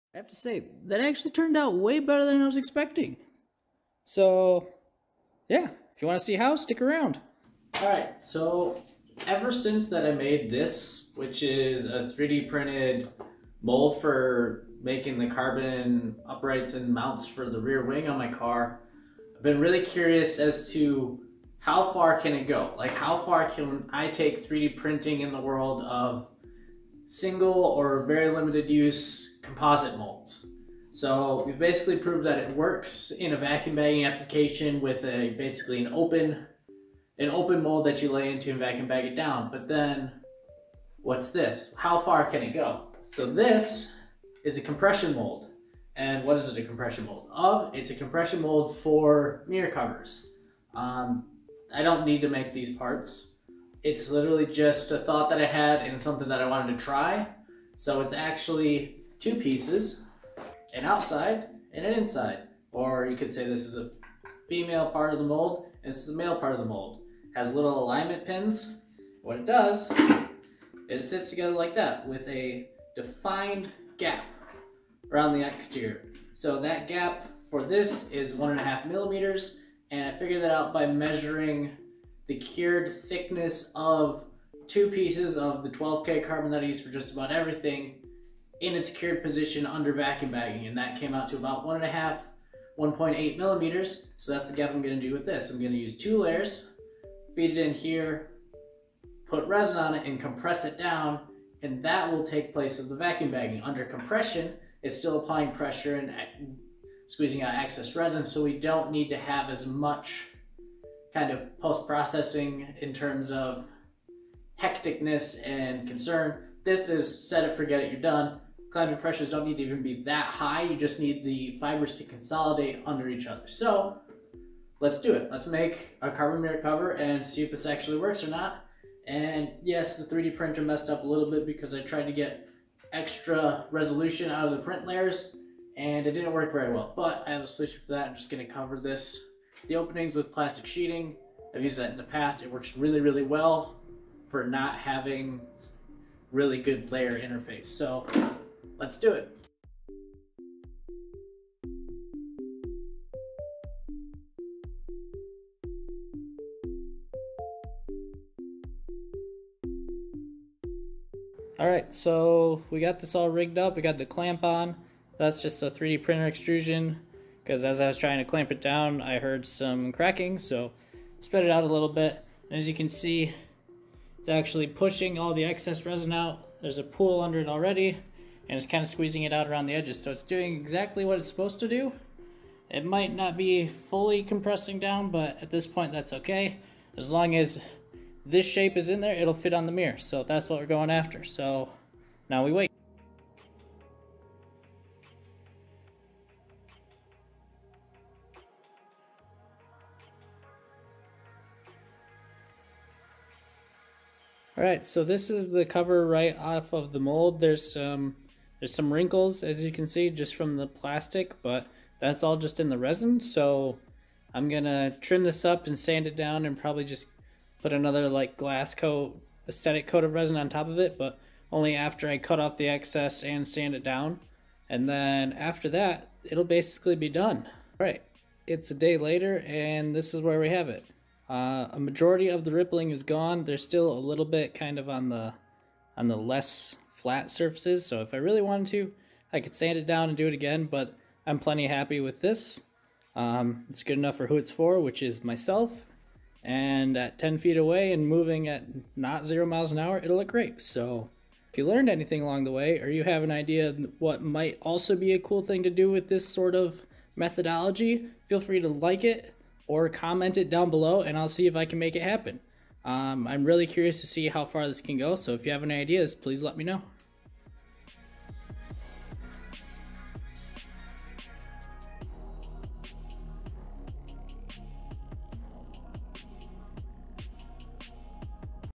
3D Printed Compression Mold: How to
Welcome to my latest tutorial: “3D Printed Compression Mold: How To” – the ultimate guide for 3D printing enthusiasts, beginner engineers, and carbon fiber aficionados! In this video, I’ll walk you through the step-by-step process of creating a high-quality compression mold using 3D printing technology. Whether you’re just starting in 3D printing or already have some experience, this tutorial is designed to help you master the techniques needed to create your own custom compression molds for composite materials like carbon fiber.

Learn how to design the mold, select the right 3D printing materials, and prepare the mold for use in carbon fiber manufacturing. I’ll also cover tips for achieving the best print quality, avoiding common mistakes, and how to integrate carbon fiber into your mold for stronger, lightweight parts.

If you’re passionate about 3D printing, composite materials, or carbon fiber, this is the perfect video to expand your skillset. Ideal for hobbyists, engineers, or anyone learning about advanced materials, this tutorial will give you the foundation to create durable, high-performance parts for a variety of applications.

Chapters:
0:00 - 0:07 First Impressions
0:07 - 0:42 Background and Intro
0:42 - 2:28 Mold Design
2:28 - 3:12 Part Layup
3:12 - 3:24 De-moulding
3:24 - 3:47 First Looks
3:47 - 4:38 Final Thoughts and Outro